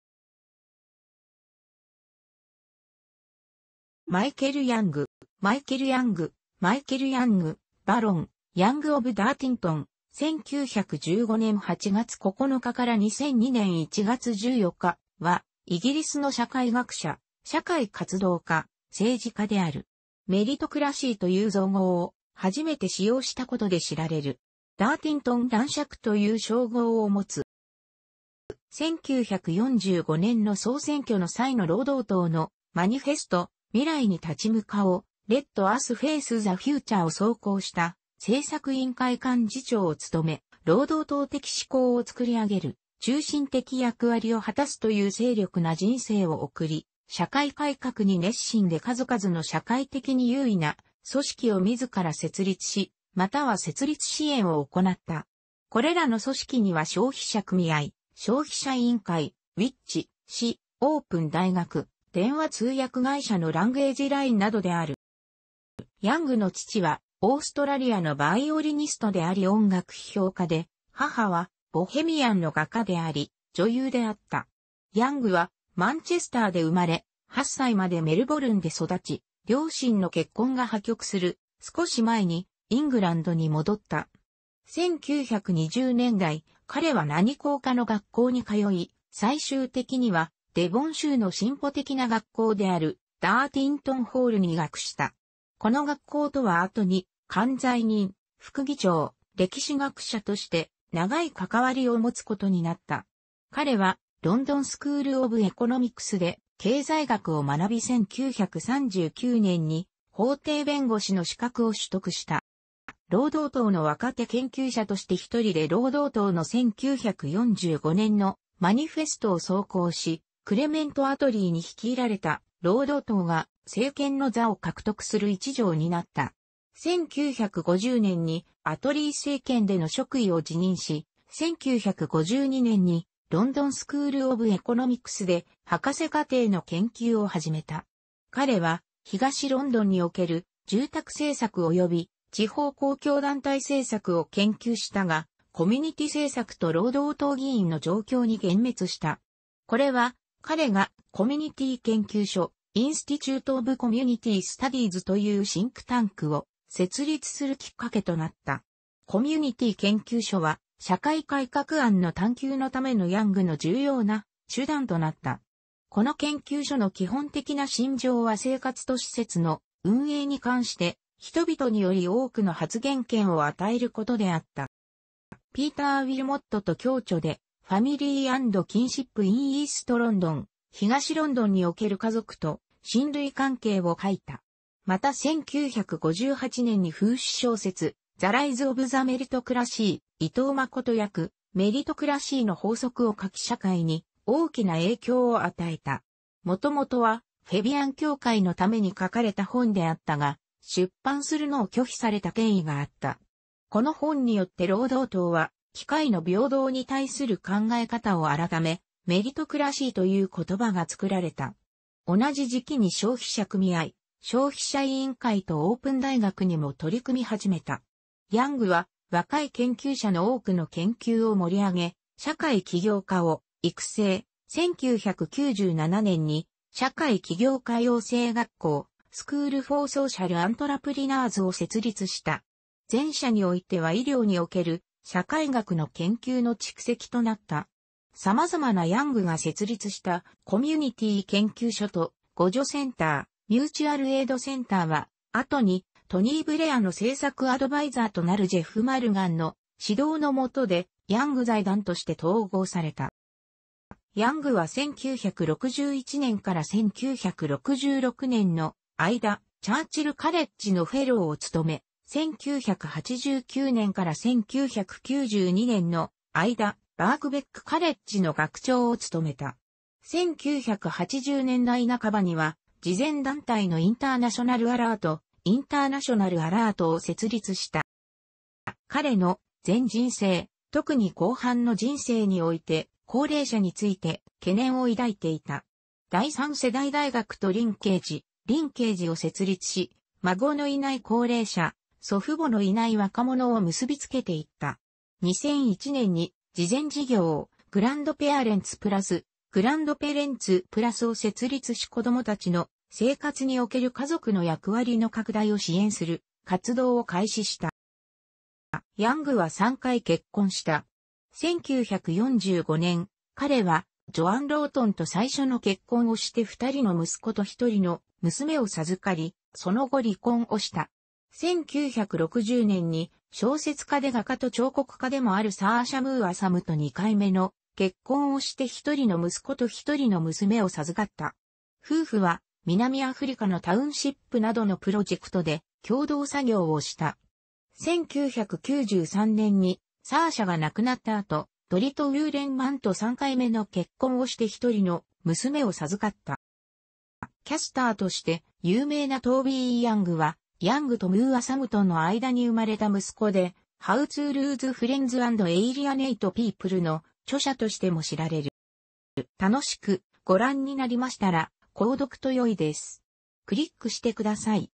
マイケル・ヤング (社会学者)
マイケル・ヤング（Michael Young, Baron Young of Dartington、1915年8月9日 - 2002年1月14日）はイギリスの社会学者、社会活動家、政治家である。メリトクラシーと言う造語を初めて使用したことで知られる。ダーティントン男爵と言う称号を持つ。

1945年の総選挙の際の労働党のマニフェスト「未来に立ち向かおう（Let Us Face the Future）」を草稿した政策委員会幹事長を務め、労働党的思考を作り上げる中心的役割を果たすという精力的な人生を送り、社会改革に熱心で数々の社会的に有意な組織を自ら設立し、または設立支援を行なった。これらの組織には消費者組合、消費者委員会、「Which?」誌、オープン大学、電話通訳会社のランゲージラインなどである。

ヤングの父はオーストラリアのヴァイオリニストであり音楽批評家で、母はボヘミアンの画家であり女優であった。ヤングはマンチェスターで生まれ、8歳までメルボルンで育ち、両親の結婚が破局する少し前にイングランドに戻った。1920年代、彼は何校かの学校に通い、最終的にはデヴォン州の進歩的な学校であるダーティントン・ホールに入学した。この学校とは後に管財人、副議長、歴史学者として長い関わりを持つことになった。彼はロンドン・スクール・オブ・エコノミクスで経済学を学び1939年に法廷弁護士の資格を取得した。

労働党の若手研究者として一人で労働党の1945年のマニフェストを草稿し、クレメント・アトリーに率いられた労働党が政権の座を獲得する一助を担った。1950年にアトリー政権での職位を辞任し、1952年にロンドン・スクール・オブ・エコノミクスで博士課程の研究を始めた。彼は東ロンドンにおける住宅政策及び地方公共団体政策を研究したが、コミュニティ政策と労働党議員の状況に幻滅した。これは彼がコミュニティー研究所（Institute of Community Studies）というシンクタンクを設立するきっかけとなった。コミュニティー研究所は社会改革案の探求のためのヤングの重要な手段となった。この研究所の基本的な信条は生活と施設の運営に関して人々により多くの発言権を与えることであった。

ピーター・ウィルモットと共著で「Family and Kinship in East London」（東ロンドンにおける家族と親類関係）を書いた。また1958年に風刺小説「The Rise Of The Meritocracy」（伊藤慎一訳「メリトクラシーの法則」）を書き社会に大きな影響を与えた。元々はフェビアン協会のために書かれた本であったが出版するのを拒否された経緯があった。この本によって労働党は機会の平等に対する考え方を改め、メリトクラシーという言葉が作られた。同じ時期に消費者組合、消費者委員会とオープン大学にも取り組み始めた。ヤングは若い研究者の多くの研究を盛り上げ、社会起業家を育成、1997年に社会起業家養成学校（School for Social Entrepreneurs）を設立した。前者においては医療における社会学の研究の蓄積となった。様々なヤングが設立したコミュニティー研究所と互助センター（Mutual Aid Centre）は、後にトニー・ブレアの政策アドバイザーとなるジェフ・マルガンの指導の元でヤング財団として統合された。

ヤングは1961年から1966年の間、チャーチル・カレッジのフェローを務め、1989年から1992年の間、バークベック・カレッジの学長を務めた。1980年代半ばには慈善団体のインターナショナル・アラート（International Alert）を設立した。

彼の全人生、特に後半の人生において、高齢者について懸念を抱いていた。第3世代大学とリンケージ（Linkage）を設立し、孫のいない高齢者、祖父母のいない若者を結びつけていった。2001年に慈善事業「グランドペアレンツ・プラス（Grandparents Plus）」を設立し子供たちの生活における家族の役割の拡大を支援する活動を開始した。

ヤングは3回結婚した。1945年、彼はジョアン・ロートンと最初の結婚をして2人の息子と1人の娘を授かり、その後離婚をした。1960年に小説家で画家と彫刻家でもあるサーシャ・ムーアサムと2回目の結婚をして一人の息子と一人の娘を授...